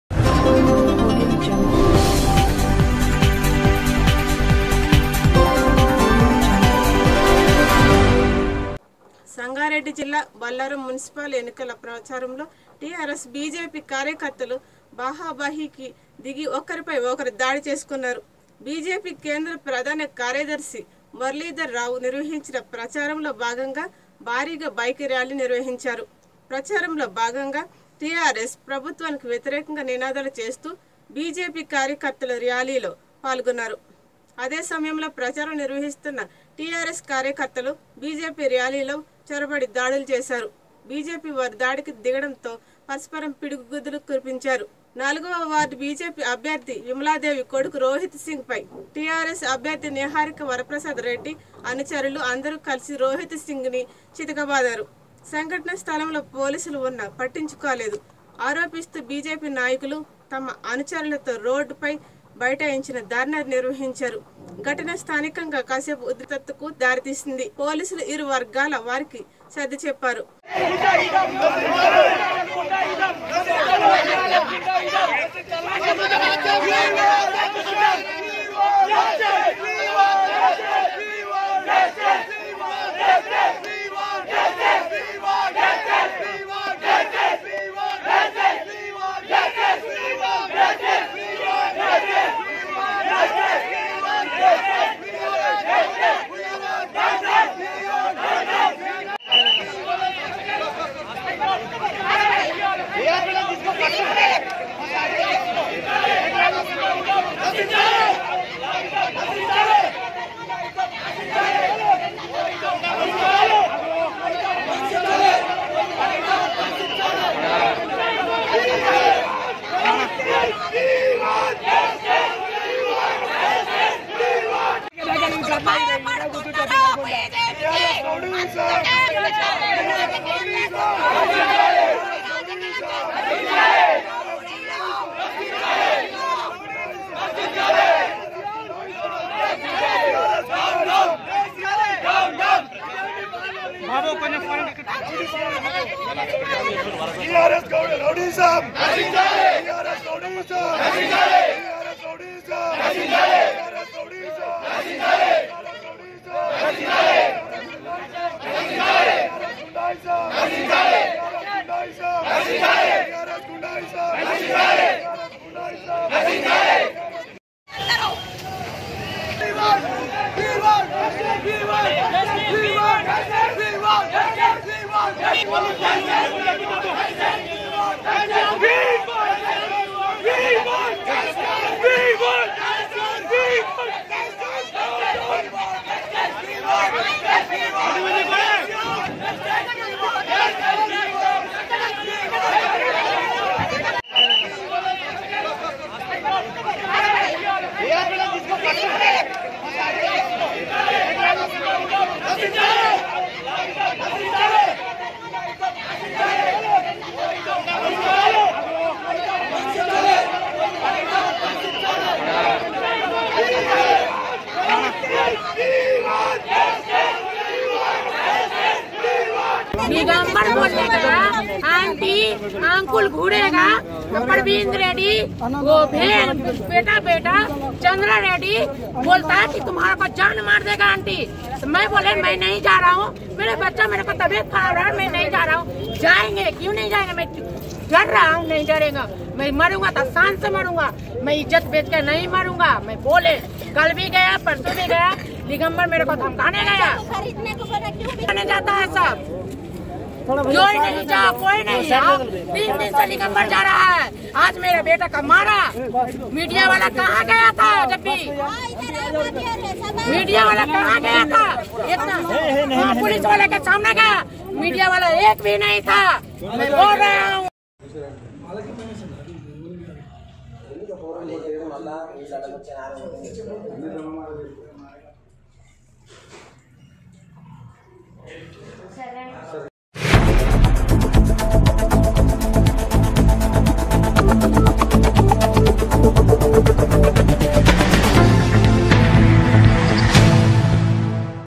సంగారెడ్డి జిల్లా బొల్లారం మున్సిపల్ ఎన్నికల ప్రచారంలో టిఆర్ఎస్,బీజేపీ కార్యకర్తల మధ్య దాడి
సంగారెడ్డి జిల్లా బొల్లారం మున్సిపల్ ఎన్నికల ప్రచారంలో టిఆర్ఎస్ బీజేపీ కార్యకర్తలు బహాబహి కి దిగి ఒక్కరిపై ఒక్కరూ దాడి చేసుకున్నారు.. బీజేపీ కేంద్ర ప్రధాన కార్యదర్శి మురళీధర్ రావు నిర్వహించిన ప్రచారంలో భాగంగా భారీగా బైక్ ర్యాలీ నిర్వహించారు,  ప్రచారంలో భాగంగా టీఆరెఎస్ ప్రభుత్వానికి వ్యతిరేకంగా నినాదాలు చేస్తూ బీజేపీ కార్యకర్తలు ర్యాలీ లో పాల్గొన్నారు.. అదే సమయంలో ప్రచారం నిర్వహిస్తున్న టీఆరెఎస్ కార్యకర్తలు బీజేపీ ర్యాలీ లో చొరబడి దాడులు చేశారు, బీజేపీ వారు దాడికి దిగడం తో పరస్పరం పిడిగుద్దులు కురిపించారు.. ప  4 వ వార్డ్ బీజేపీ అభ్యర్థి విమలదేవి  కొడుకు రోహిత్ సింగ్ పై టీఆరెఎస్ అభ్యర్థి నీహారిక వరప్రసాద్ రెడ్డి అనుచరులు  అందరూ కలిసి రోహిత్ సింగ్ నీ చితకబాదారు.. సంఘటన స్థలంలో పోలీసులు వున్న పటించుకొలేదు ఆరోపిస్తూ  బీజేపీ నాయకులు, తమ అనుచరులతో రోడ్డు పై బైఠాయించి ధర్నా నిర్వహించారు... ఘటన స్థానికంగా కాసేపు ఉద్రిక్తతకు దారితీసింది.. పోలీసులు ఇరు వర్గాల వారికి సర్ది చెప్పారు...